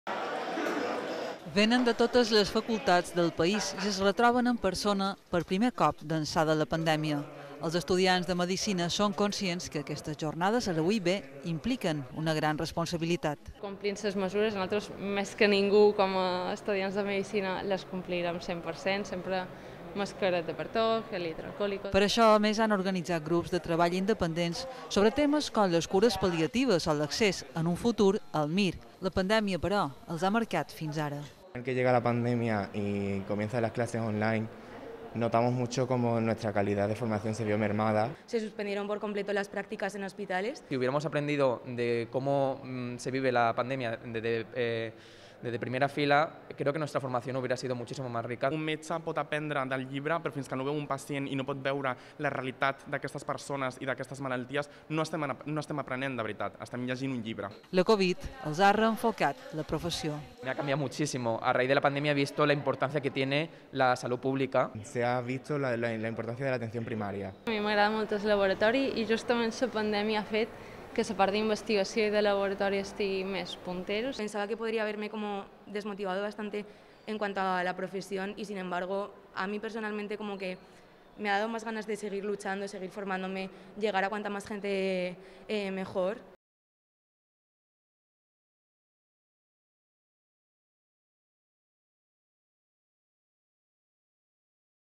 80 representants de tot Espanya participen en la XCI Jornades del Consell Estatal d'Estudi
És la primera trobada presencial d'ençà de la pandèmia, que està marcant la seva formació